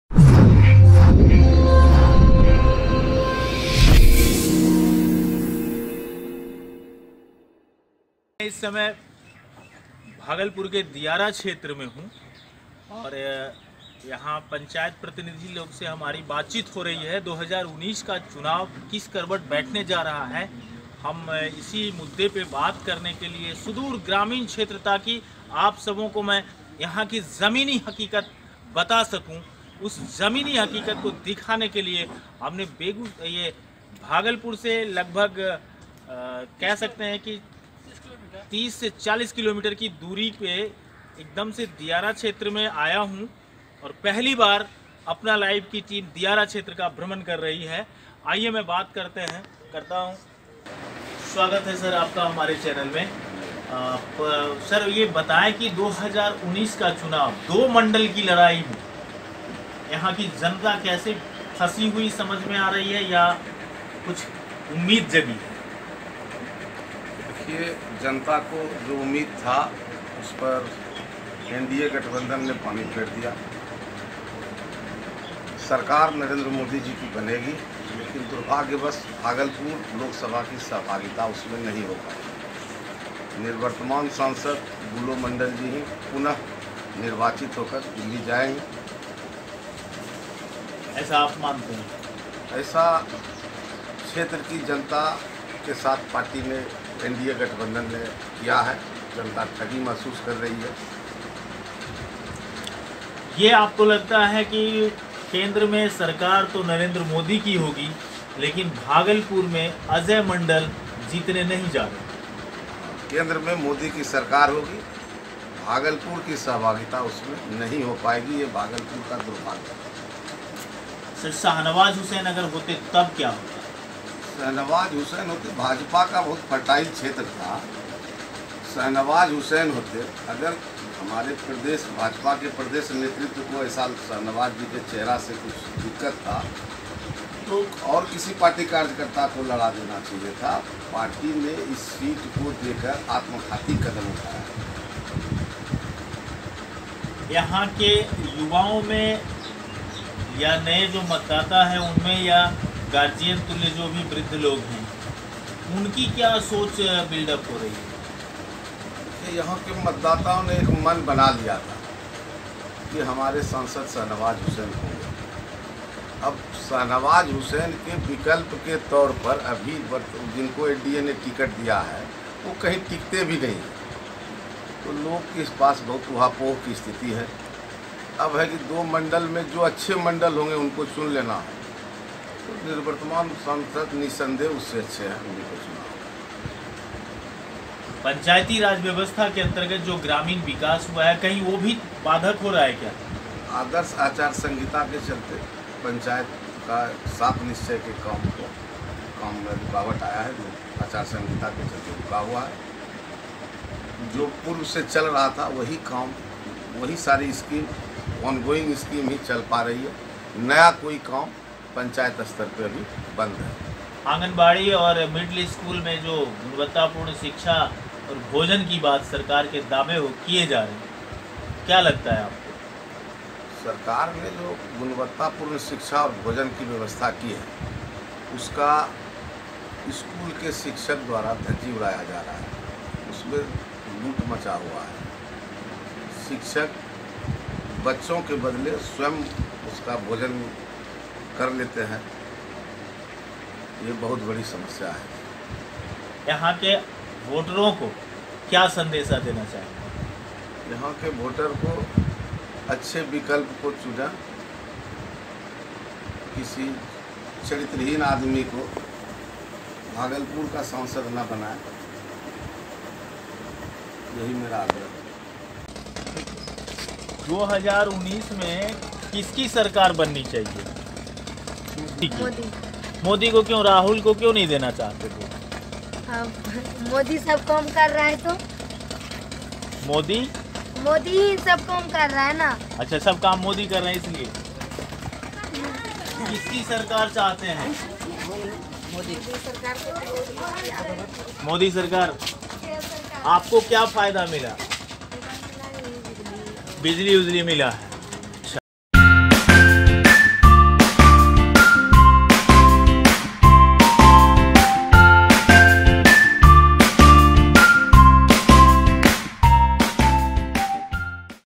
भागलपुर के दियारा क्षेत्र के आम लोग क्या कहते हैं 2019 के चुनाव को ले कर ।
नमस्कार आज हम लोग इस वीडियो में देखेंगे कि
भागलपुर के दियारा क्षेत्र के आम लोगो का क्या कहना हैं 2019 के
लोकसभा चुनाव को लेकर।वीडियो पर क्लिक कर के जरूर देखें।

By:-Pankaj Bhatt